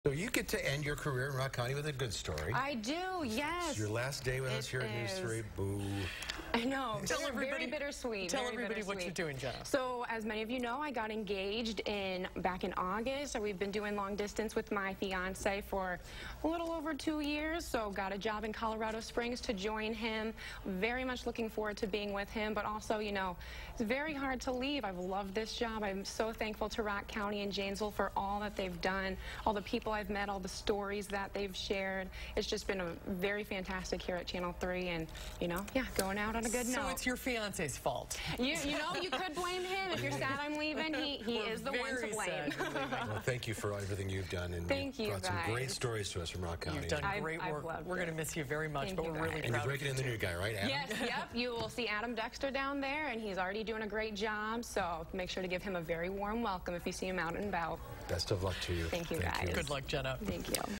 News 3 says goodbye to Rock County reporter Jenna Middaugh
Rock County reporter Jenna Middaugh says goodbye to News 3 before she moves to Colorado Springs.